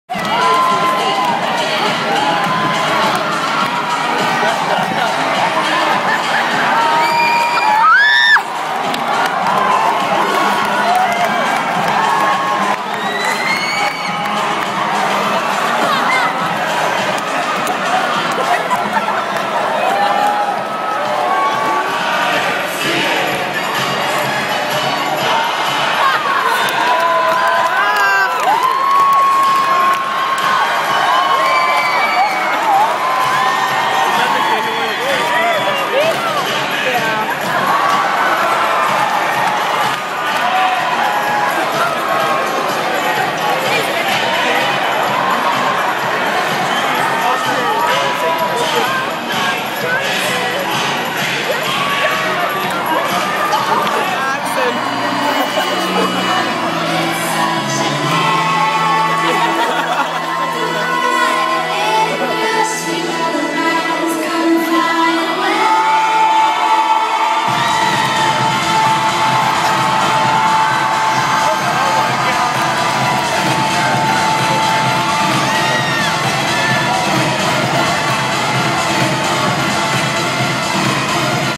typical day at Allen High School
our very own Napoleon Dynamite and then dance off, then after the 3 hours they locked all the seniors in the gym, they sent us to lunch and there was a food fight, not captured :(